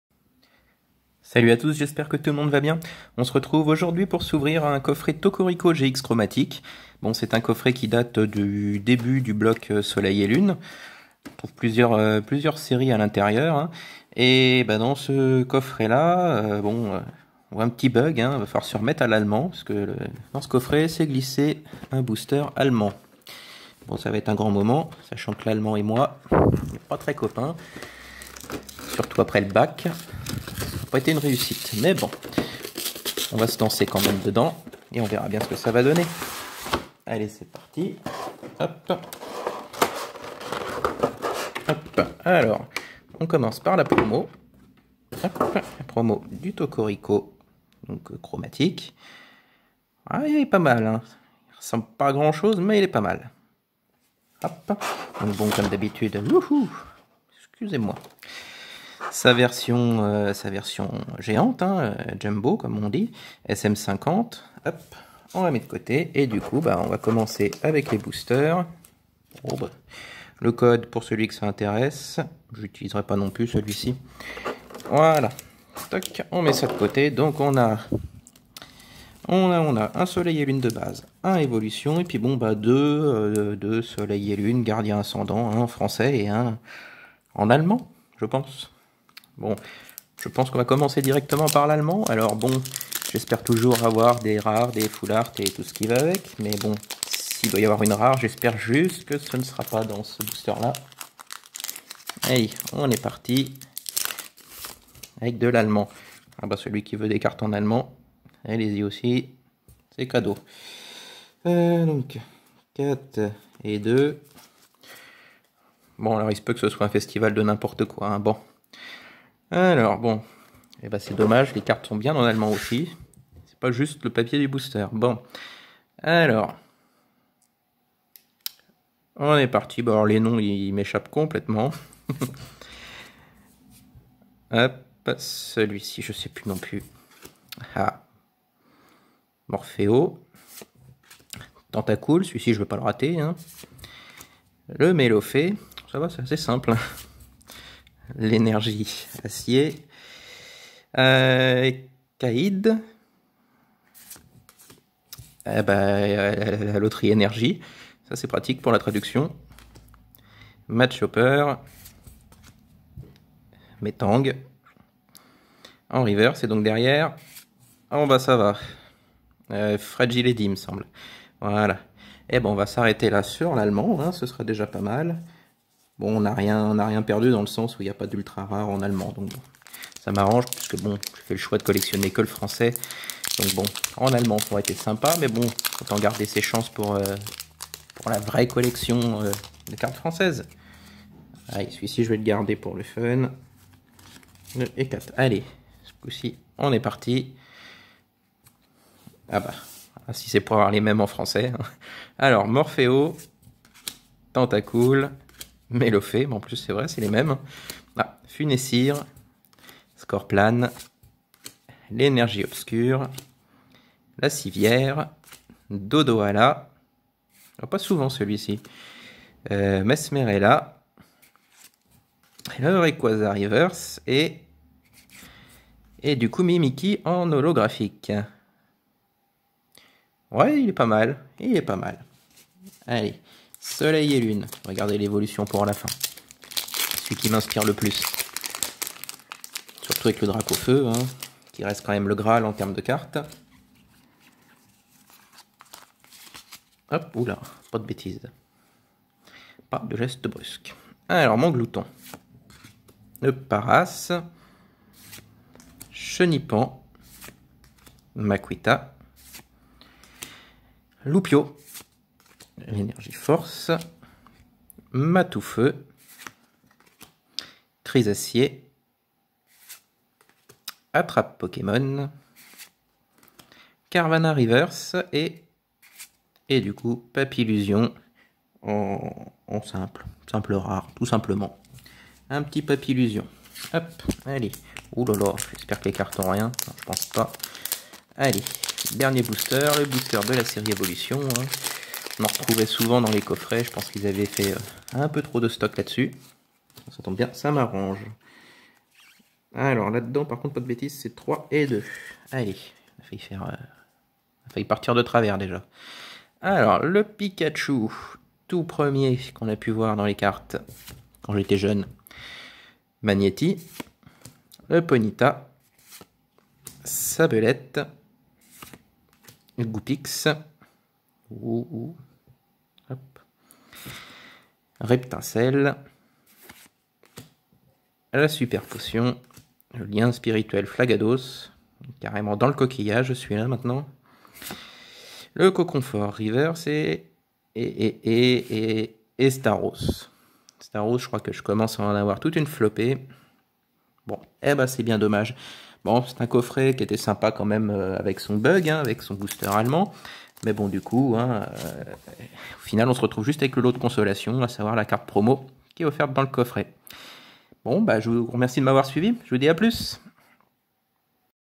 # Coffret Bug # Ouverture du coffret Tokorico-GX Chromatique
Ouverture d'un coffret Pokémon Tokorico GX Chromatique, avec ses 4 boosters et un Bug ...